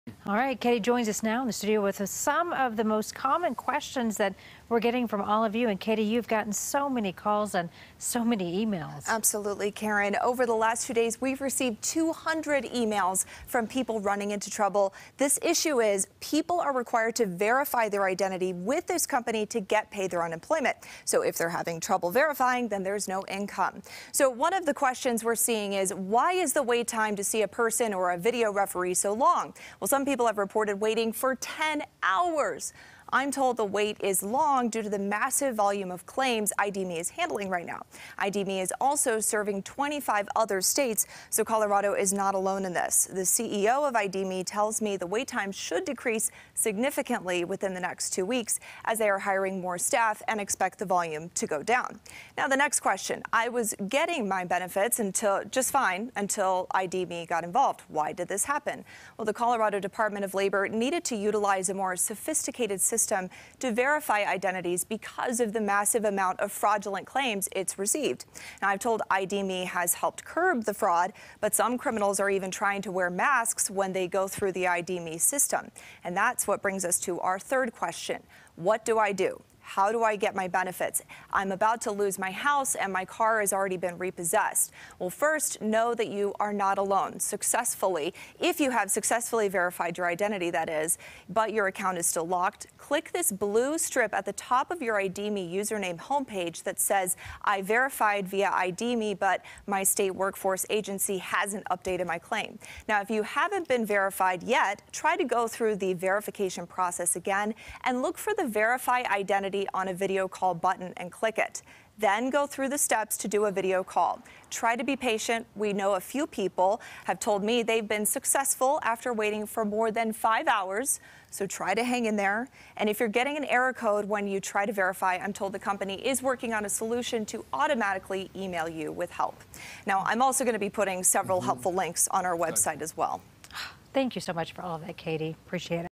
Q&A: CBS4 Investigator Kati Weis Answers Questions About ID.me Troubles
The ID.me system put in place in Colorado to try to combat fraudulent unemployment claims is putting some people who are out of work in a desperate situation.